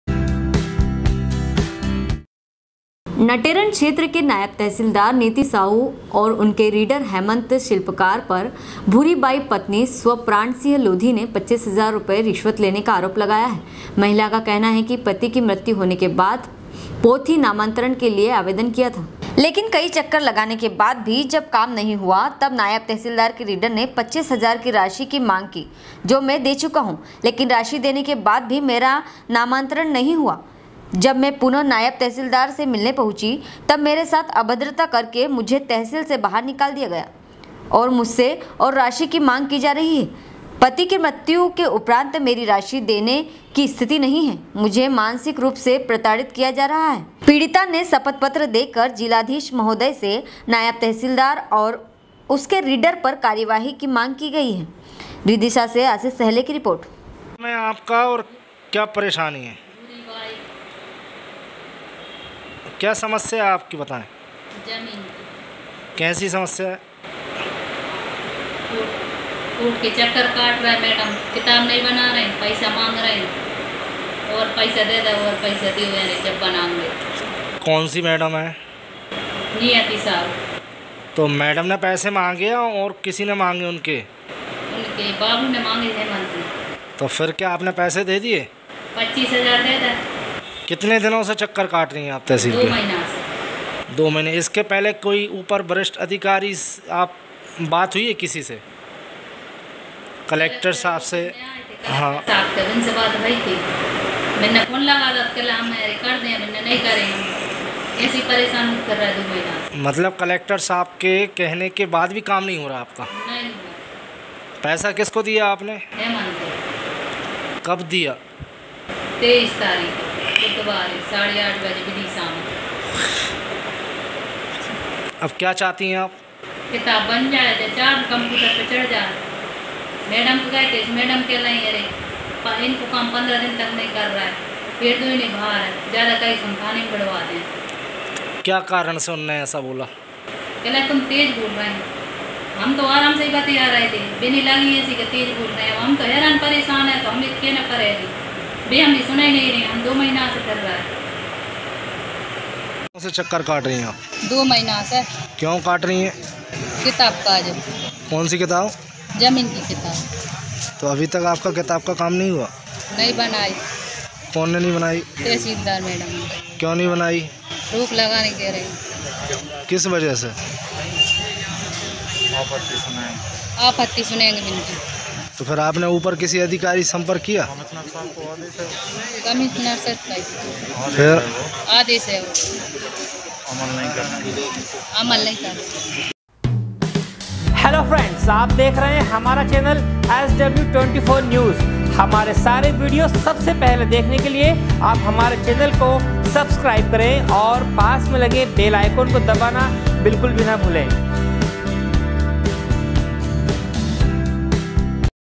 SW 24 News Madhyapradesh
नटेरन क्षेत्र के नायब तहसीलदार नियति साहू ओर उनके रीडर हेमंत शिल्पकार पर भूरीबाई पत्नी स्व प्राण सिंह लोधी ने 25 हजार रुपये रिश्वत लेने का आरोप लगाया है। महिला का कहना है कि पति की मृत्यु होने के बाद पोथी नामांतरण के लिये आवेदन किया था। विदिशा से आशिष सहेले की रिपोर्ट